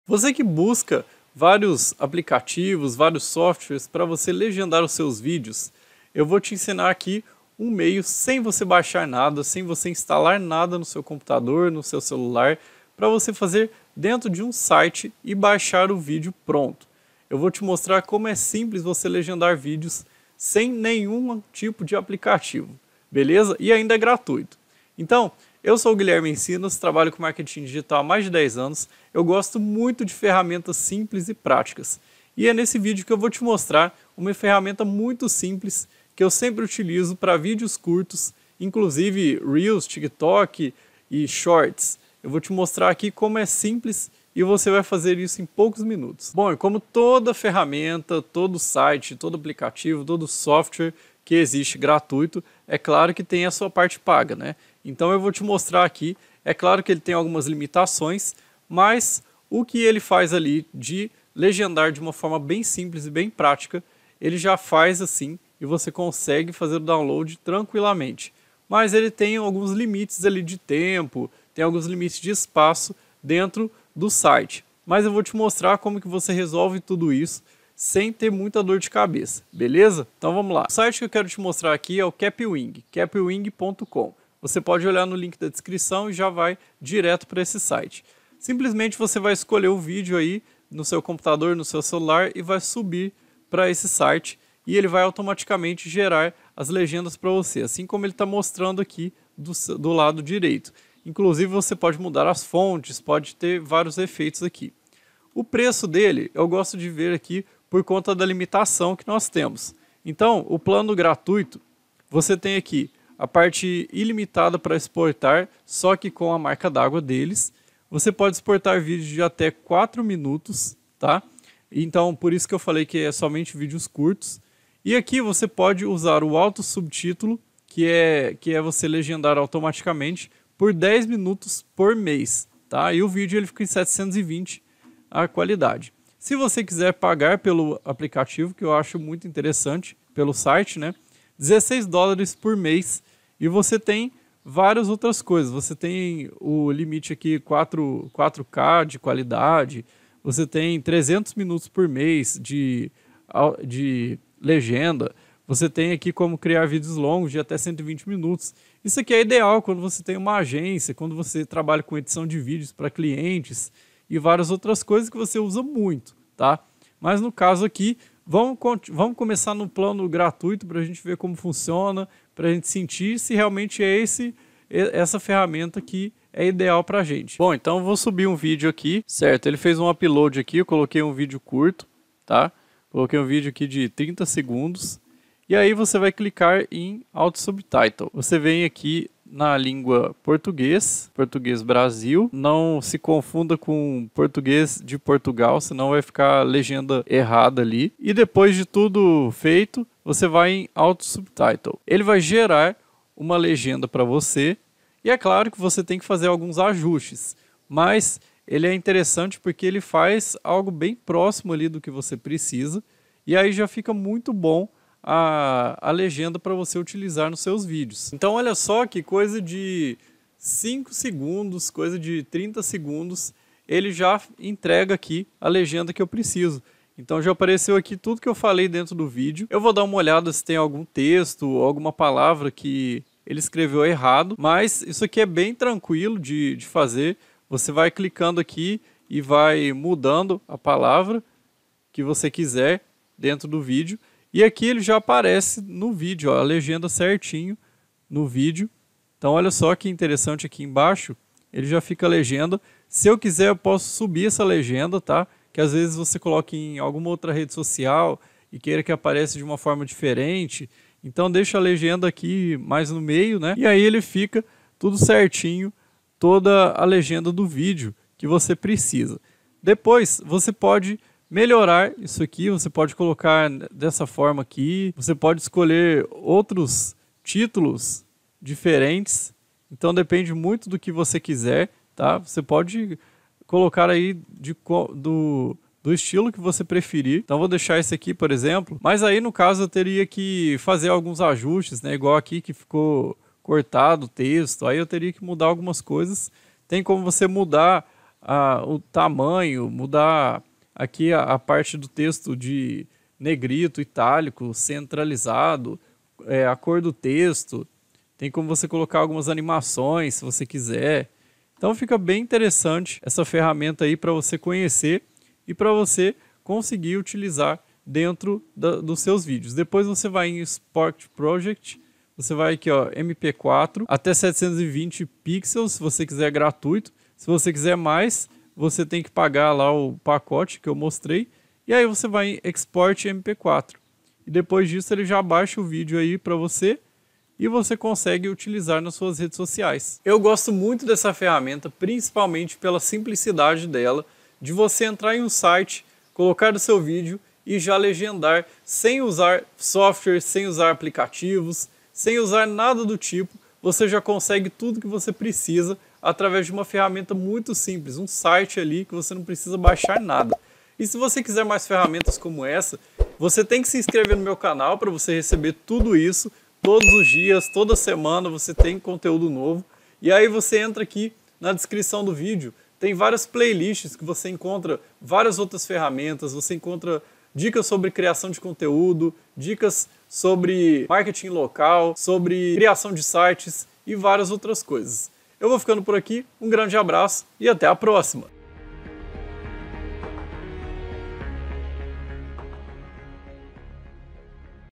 COMO LEGENDAR VÍDEOS AUTOMATICAMENTE SEM USAR PROGRAMAS E APLICATIVOS | GUILHERME ENCINAS
Se você deseja legendar seus vídeos curtos para Shorts, Tiktok e Reels eu vou te mostrar um site excelente para gerar automaticamente legendas e efeitos pra você.

No plano gratuito é possível legendar vídeos de até 4 minutos e baixar com 720p de qualidade.

Você não precisa de baixar nenhum aplicativo e nem instalar nenhum programa. O Kapwing também conta com editor de vídeos dentro do site.